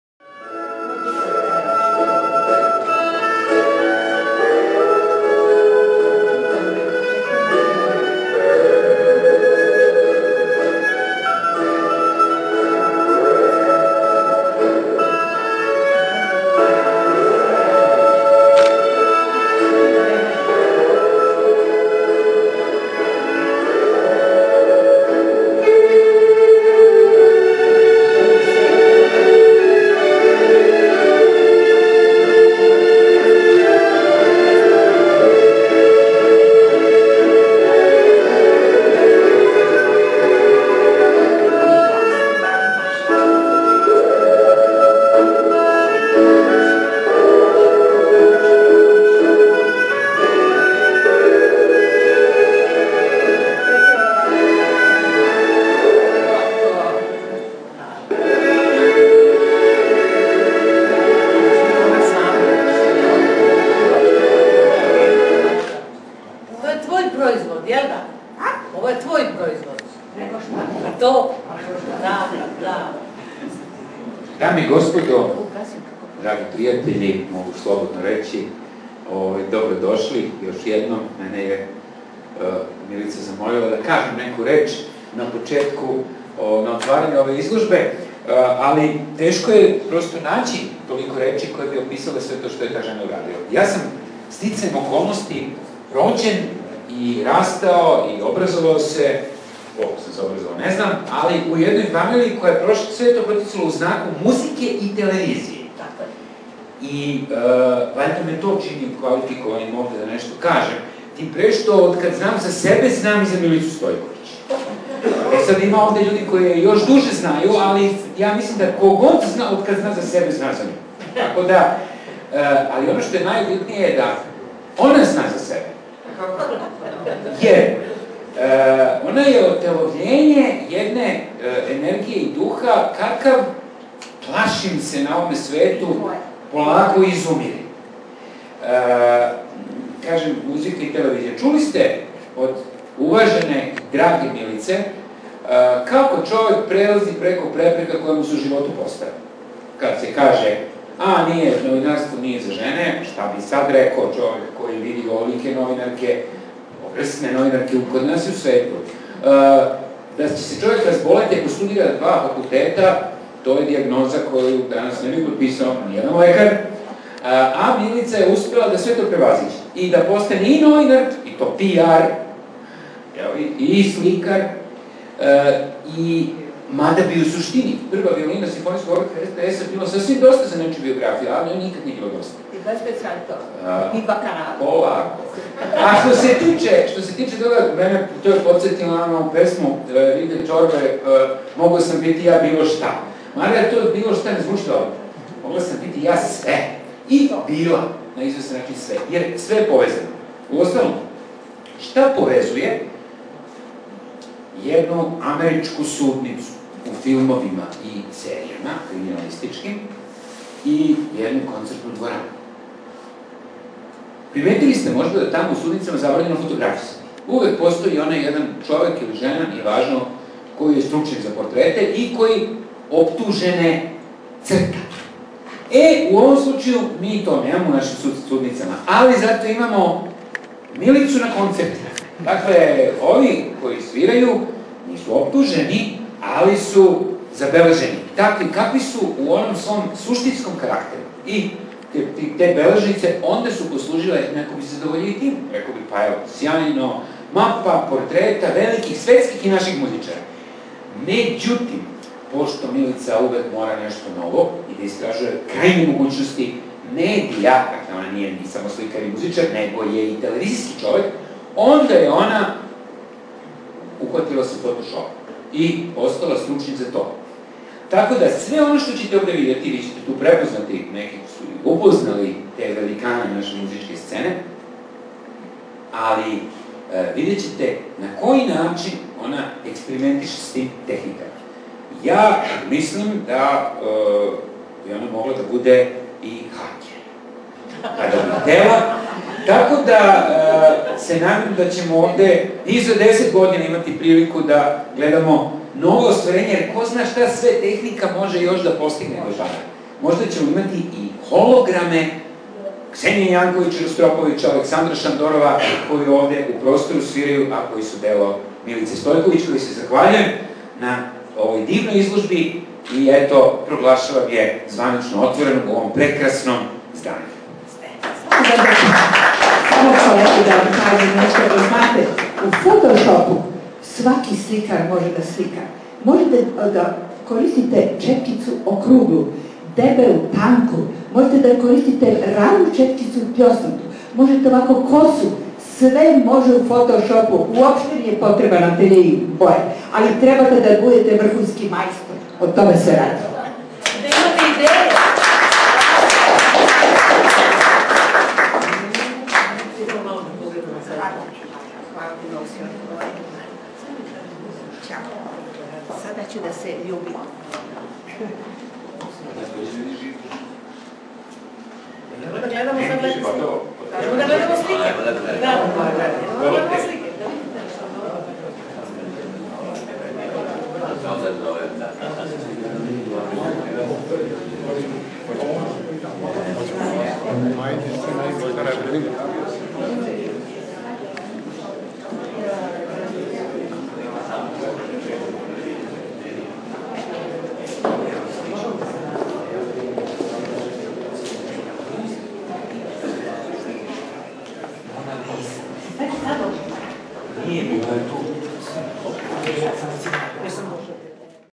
GVARNERIUS "MUZIČARI I MUZE" I OLIVER TOMIĆ
Profesor dr.Oliver Tomić otvara izložbu Milice Stojkovič "MUZIČARI I MUZE" u "GVARNERIUSU" Jovana Kolundžije.Kamera Dušan Dingarac, montaža Milica Stojković